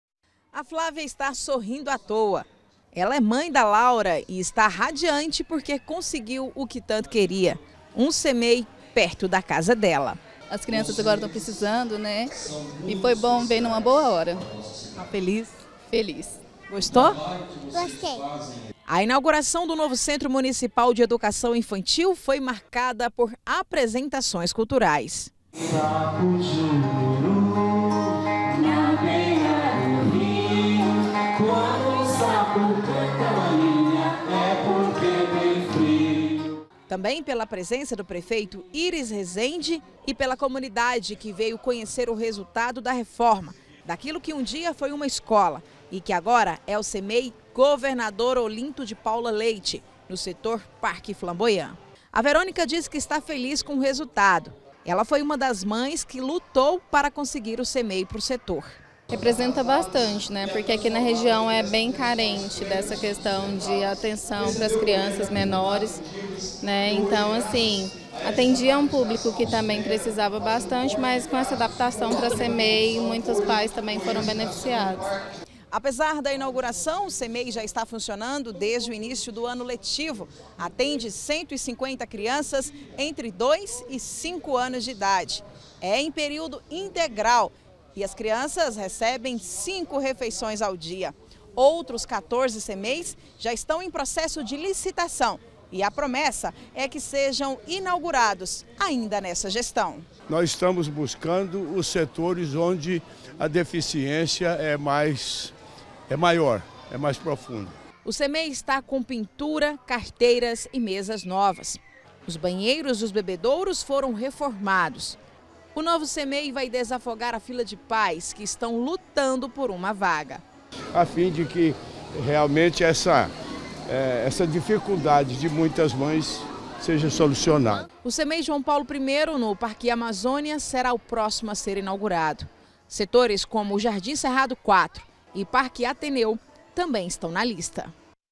EDUCAÇÃO INFANTIL: MORADORES COMEMORAM INAUGURAÇÃO DE CMEI
Os moradores do Setor Parque Flamboyant, região sul da capital, estão felizes é que hoje foi inaugurado um Centro Municipal de Educação Infantil. Quem não tinha com quem deixar os filhos pra trabalhar comemora a entrega do CMEI.